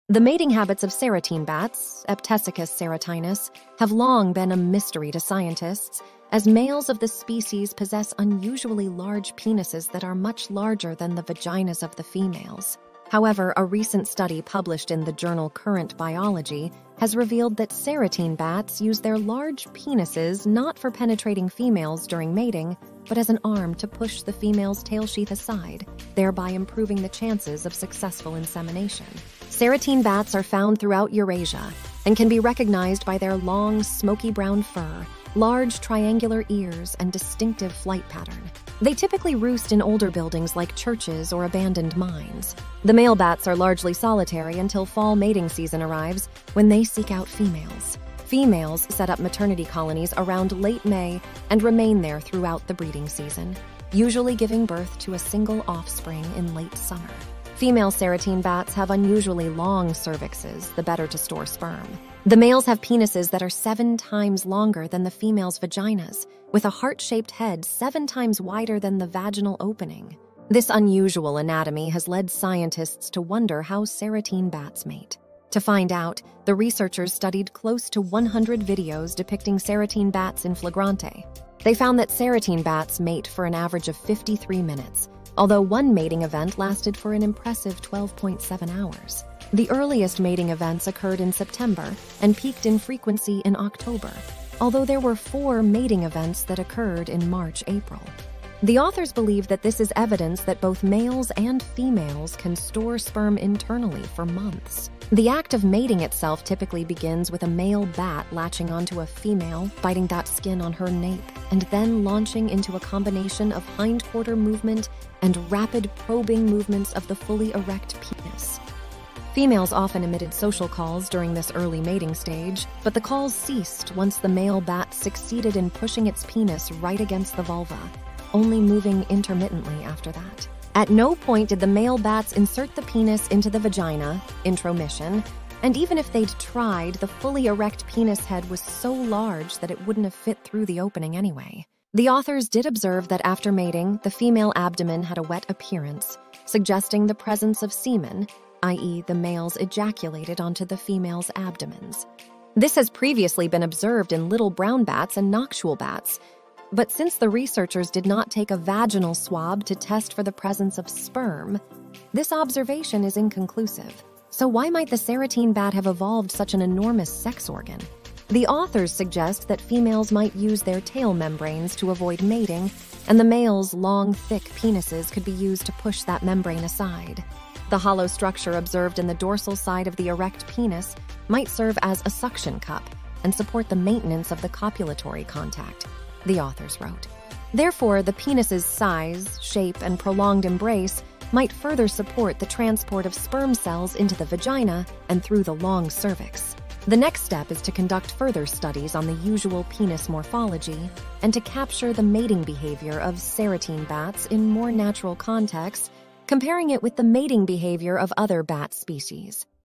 Serotine Bats Mate in a Uniquely Mammalian Way
The mating habits of serotine bats (Eptesicus serotinus) have long been a mystery to scientists, as males of the species possess unusually large penises that are much larger than the vaginas of the females. However, a recent study published in the journal Current Biology has revealed that serotine bats use their large penises not for penetrating females during mating, but as an arm to push the female's tail sheath aside, thereby improving the chances of successful insemination.

Serotine bats are found throughout Eurasia and can be recognized by their long smoky-brown fur, large triangular ears, and distinctive flight pattern. They typically roost in older buildings like churches or abandoned mines. The male bats are largely solitary until fall mating season arrives, when they seek out females. Females set up maternity colonies around late May and remain there throughout the breeding season, usually giving birth to a single offspring in late summer.

Female serotine bats have unusually long cervixes, the better to store sperm. The males have penises that are seven times longer than the females' vaginas, with a heart-shaped head seven times wider than the vaginal opening. This unusual anatomy has led scientists to wonder how serotine bats mate.

To find out, the researchers studied close to 100 videos depicting serotine bats in flagrante. They found that serotine bats mate for an average of 53 minutes, although one mating event lasted for an impressive 12.7 hours. The earliest mating events occurred in September and peaked in frequency in October, although there were four mating events that occurred in March/April. The authors believe that this is evidence that both males and females can store sperm internally for months.

The act of mating itself typically begins with a male bat latching onto a female, biting the skin on her nape, and then launching into a combination of hindquarter movement and "rapid probing movements of the fully erect penis." Females often emitted social calls during this early mating stage, but the calls ceased once the male bat succeeded in pushing its penis right against the vulva, only moving intermittently after that. At no point did the male bats insert the penis into the vagina (intromission), and even if they'd tried, the fully erect penis head was so large that it wouldn't have fit through the opening anyway.

The authors did observe that after mating, the female abdomen had a wet appearance, suggesting the presence of semen (i.e., the males ejaculated onto the females' abdomens). This has previously been observed in little brown bats and noctule bats, but since the researchers did not take a vaginal swab to test for the presence of sperm, this observation is inconclusive.

So why might the serotine bat have evolved such an enormous sex organ? The authors suggest that females might use their tail membranes to avoid mating, and the males' long, thick penises could be used to push that membrane aside. "The hollow structure observed in the dorsal side of the erect penis might serve as a suction cup and support the maintenance of the copulatory contact," the authors wrote. "Therefore, the penis's size, shape, and prolonged embrace might further support the transport of sperm cells into the vagina and through the long cervix."

The next step is to conduct further studies on the usual penis morphology and to capture the mating behavior of serotine bats in more natural contexts, comparing it with the mating behavior of other bat species.